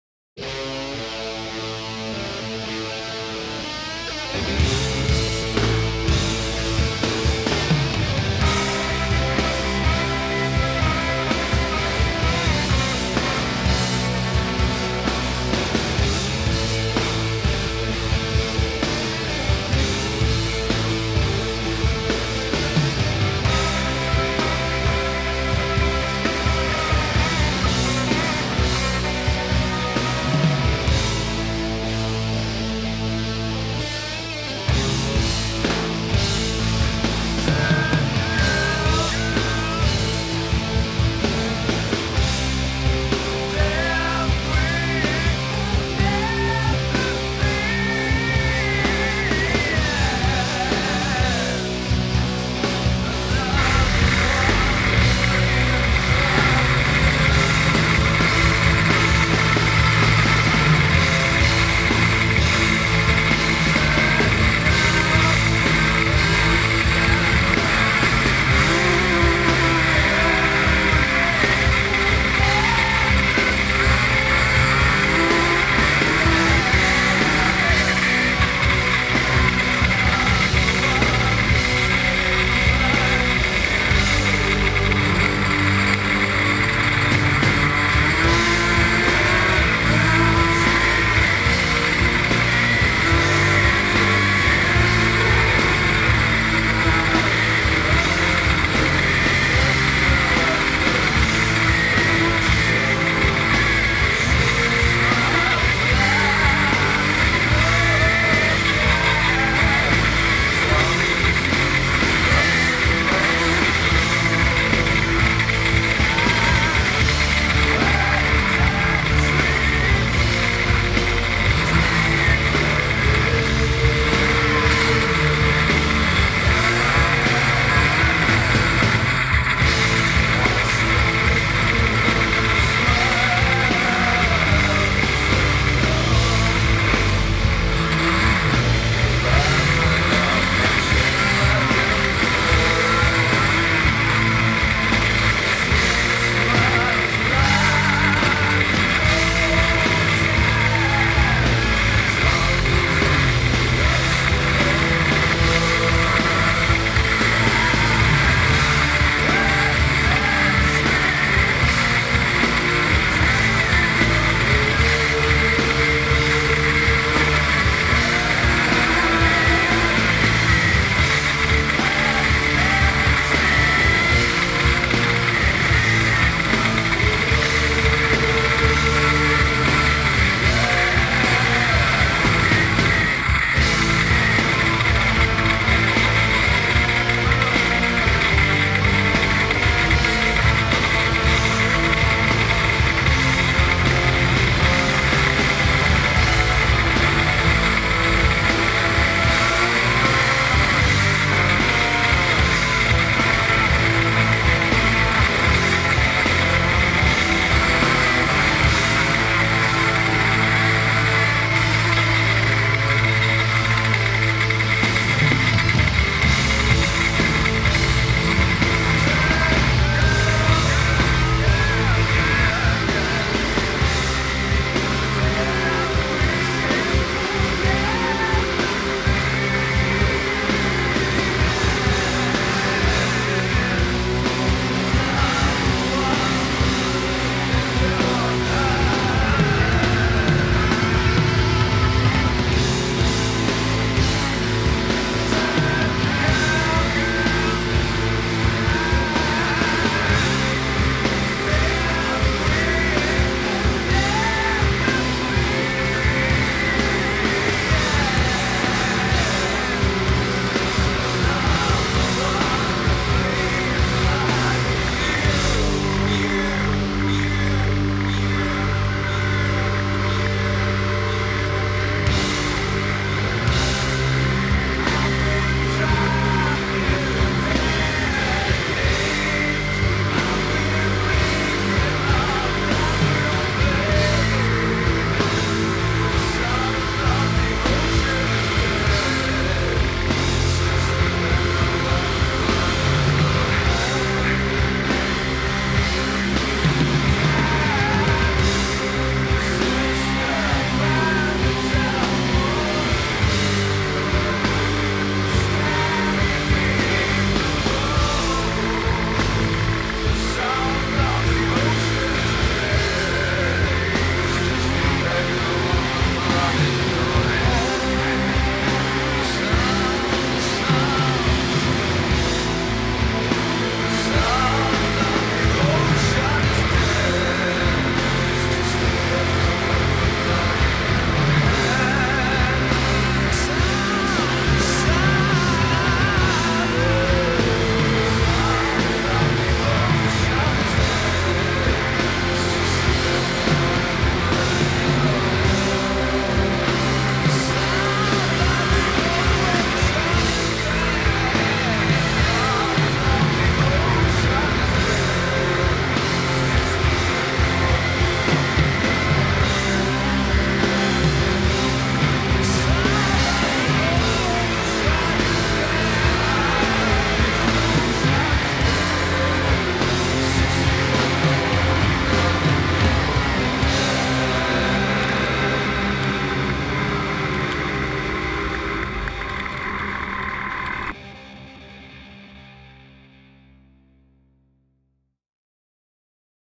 My 85.5 Yamaha Tri-z ytz250+pov riding video
some photos of my trike and some video..